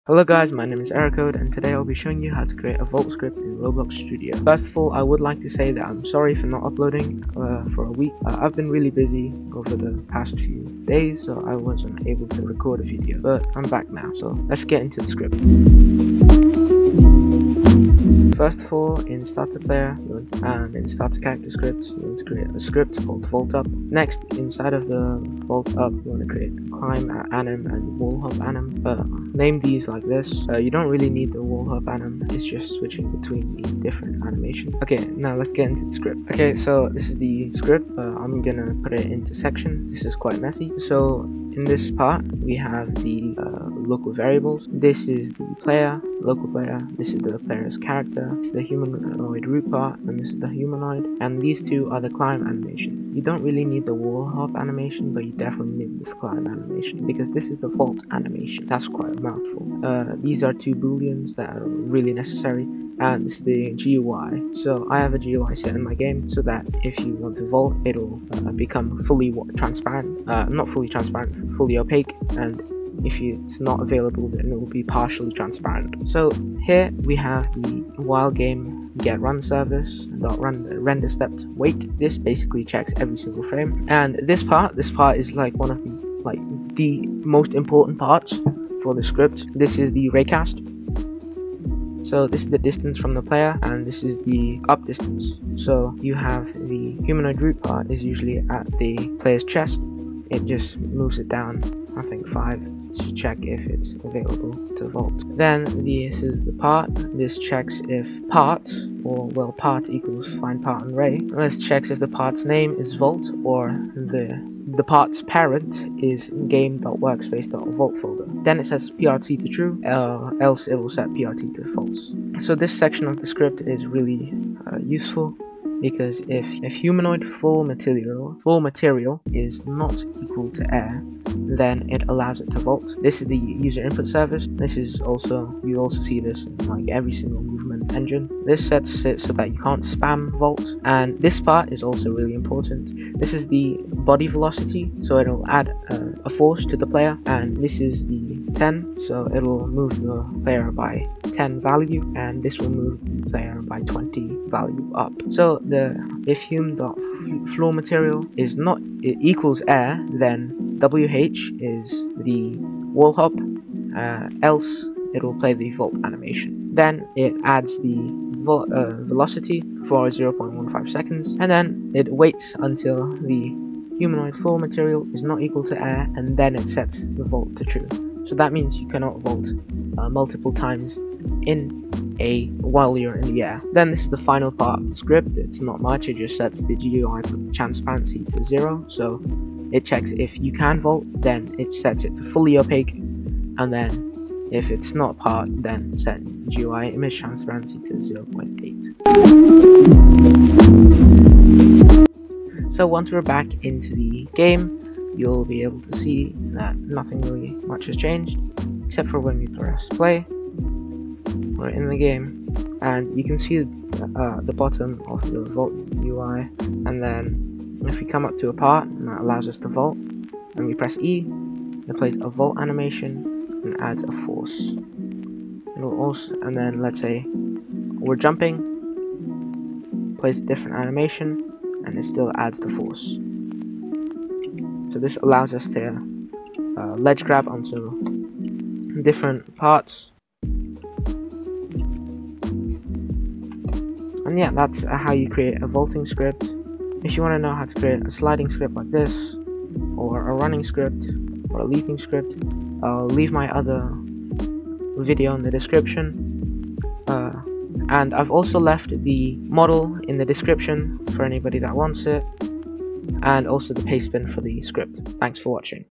DEVLOG: How to make a Vaulting System
In todays tutorial Ill show you guys how to make a movement system!

Hang out in my Discord!

TAGS